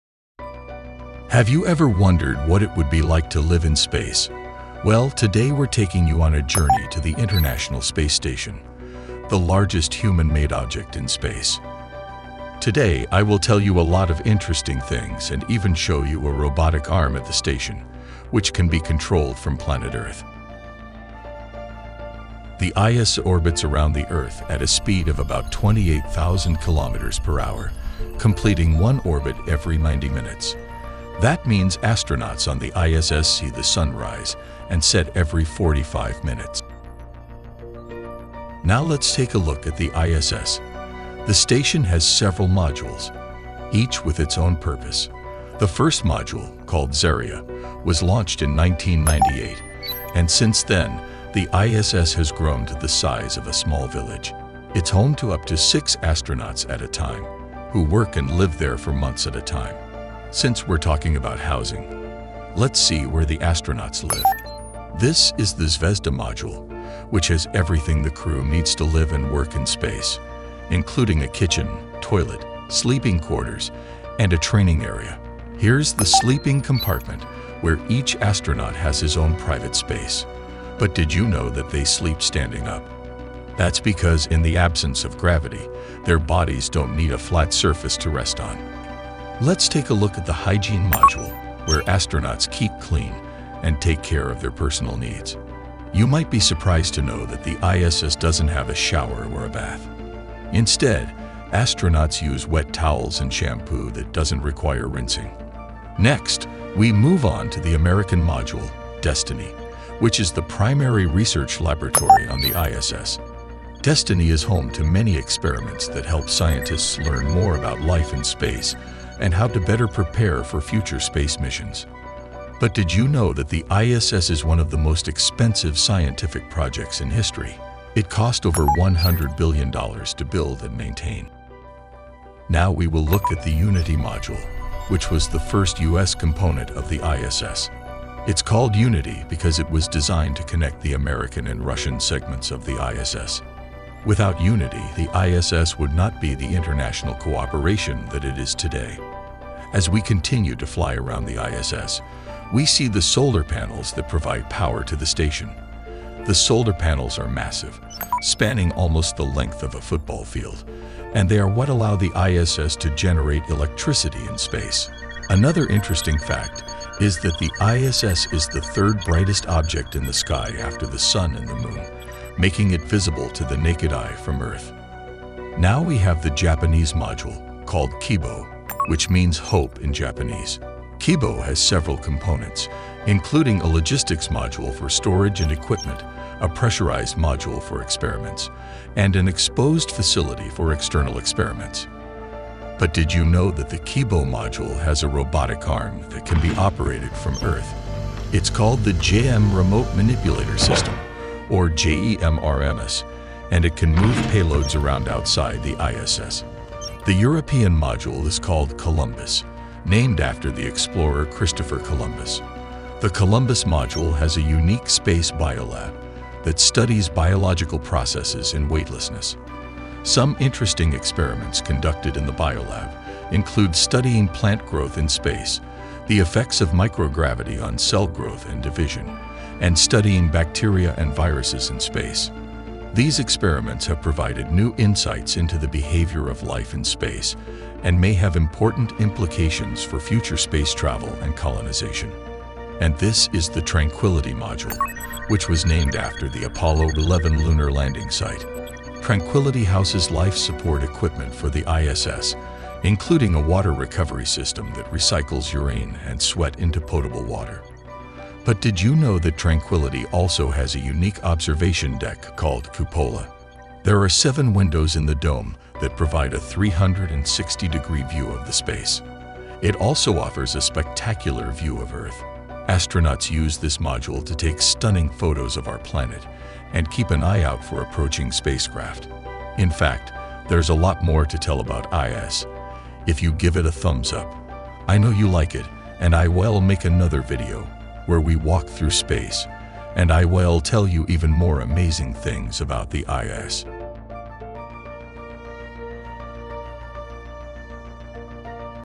How the Space Station works. Discover the International Space Station in 3D animation
Join us on an incredible journey to the International Space Station, the largest human-made object in space! Orbiting the Earth at a speed of about 28,000 kilometers per hour, the ISS completes one orbit every 90 minutes, providing its inhabitants with a unique view of the sun rising and setting every 45 minutes.

In this video, we take a close look at the various modules of the ISS, including Zarya, Zvezda, Destiny, Unity, Kibo, Columbus, and Tranquility. Learn about the living quarters, exercise equipment, hygiene facilities, and research labs available on board, all designed to support the crew during their extended stay in space.

Discover how astronauts maintain their muscle and bone mass in zero gravity, and how they stay clean and take care of their personal needs. Witness the amazing view of Earth from the Cupola observation deck and learn how the solar panels spanning almost the length of a football field generate electricity in space.

We also highlight some fascinating facts about the ISS, such as its cost of over 100 billion dollars, making it one of the most expensive scientific projects in history. We also showcase the JEM Remote Manipulator System, a robotic arm in the Kibo module that can be operated from Earth, and the unique space Biolab in Columbus that studies biological processes in weightlessness.

Don't miss this incredible opportunity to explore the International Space Station, the ultimate symbol of human innovation and collaboration in space!